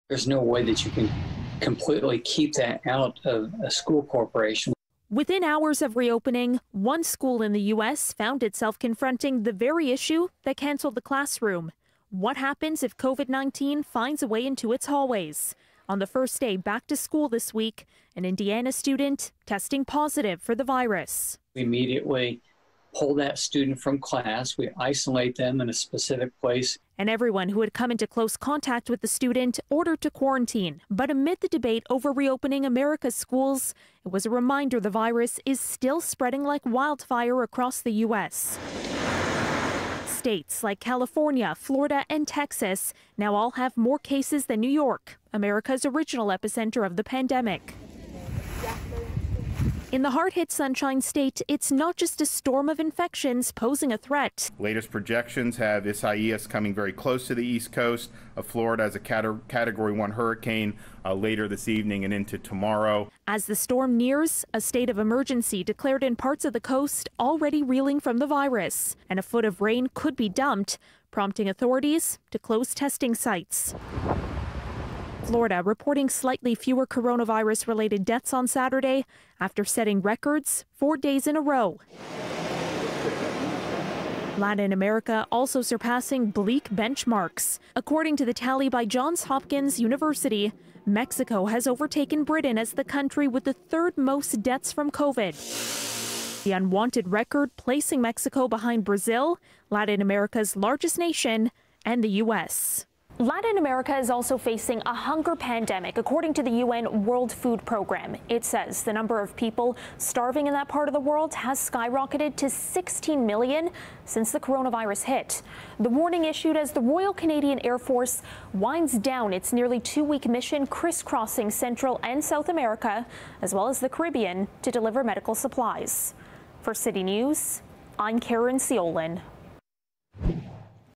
Indiana student tests positive on 1st day back to school
A student in the U.S. tests positive for coronavirus on the first day back to school. Caryn Ceolin with the other bleak benchmarks being passed, as the coronavirus continues to spread like wildfire in parts of the world.